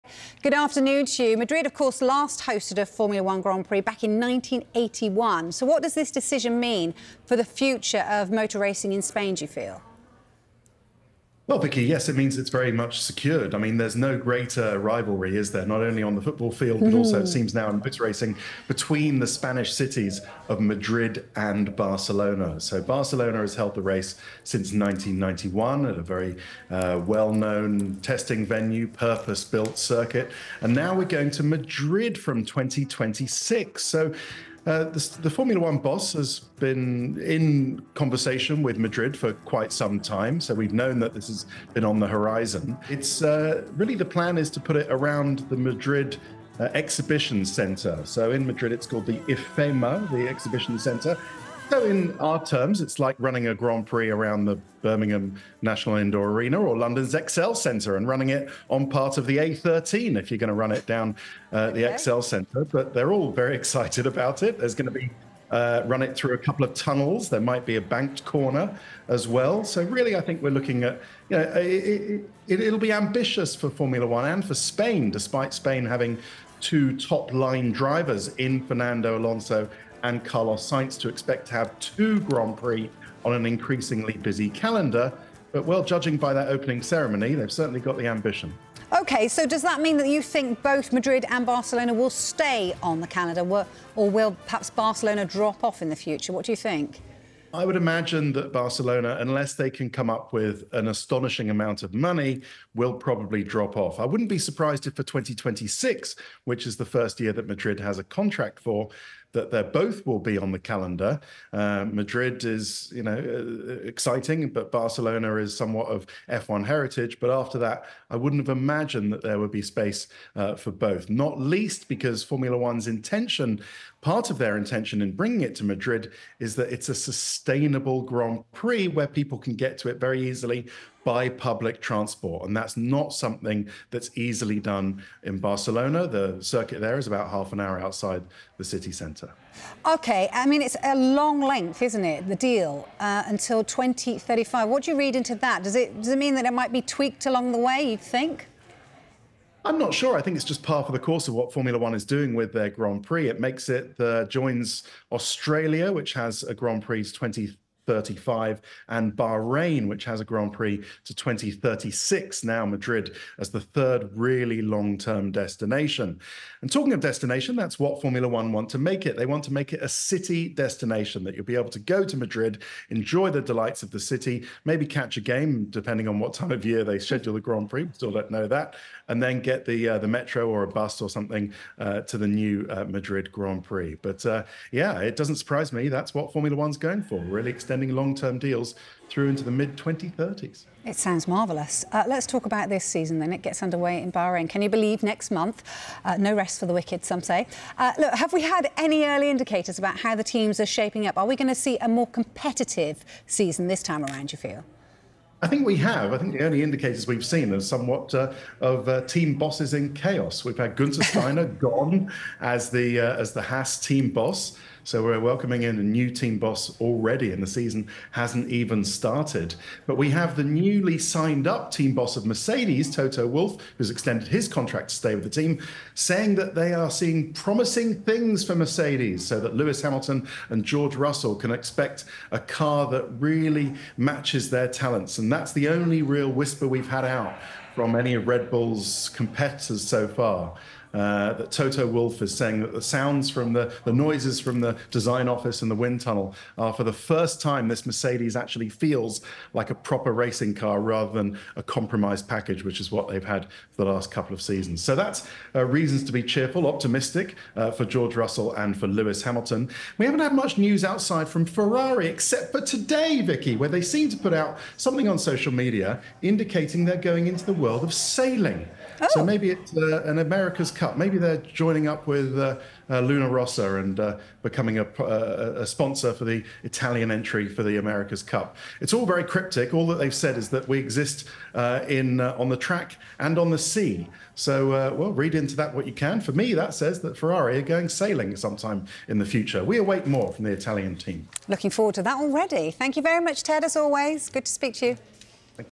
What's the future for the Spanish Grand Prix? 💭| Ted Kravitz on Madrid joining F1 from 2026! 📅
Ted Kravitz gives his thoughts on the Spanish Grand Prix after it was announced that Madrid would join the F1 calendar from 2026.